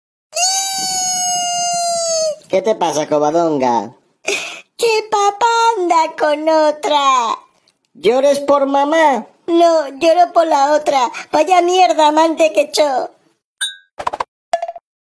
El padre de Covadonga ye infiel
El padre de Chusín y Covadonga anda con otra